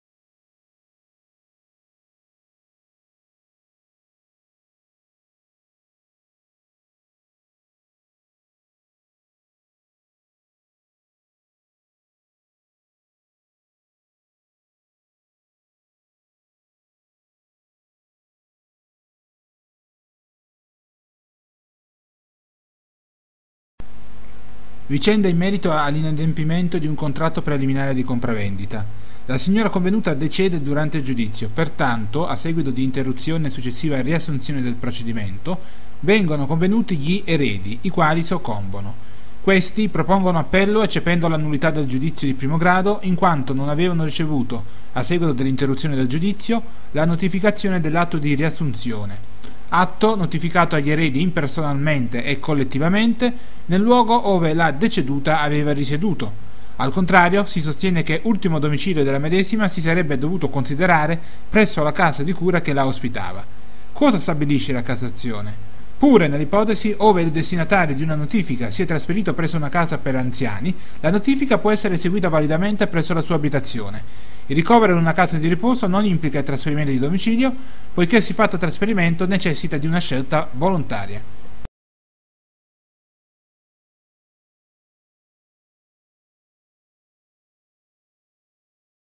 L'inadempiente muore in una casa di cura, dove va inviato l'atto di riassunzione agli eredi ?
Cassazione civile , sez. VI, ordinanza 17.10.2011 n° 21370

Vicenda in merito all'inadempimento di un contratto preliminare di compravendita.

La signora convenuta decede durante il giudizio, pertanto, a seguito di interruzione e successiva riassunzione del procedimento, vengono convenuti gli eredi, i quali soccombono. Questi propongono appello eccependo la nullità del giudizio di primo grado in quanto non avevano ricevuto, a seguito dell'interruzione del giudizio, la notificazione dell'atto di riassunzione: atto notificato agli eredi impersonalmente e collettivamente nel luogo ove la deceduta aveva risieduto; al contrario, si sostiene che ultimo domicilio della medesima si sarebbe dovuto considerare presso la casa di cura che la ospitava.

Cosa stabilisce la Cassazione ? Pure nell'ipotesi ove il destinatario di una notifica si è trasferito presso una casa per anziani, la notifica può essere eseguita validamente presso la sua abitazione: il ricovero in una casa di riposo non implica il trasferimento di domicilio poiché siffatto trasferimento necessita di una "scelta volontaria".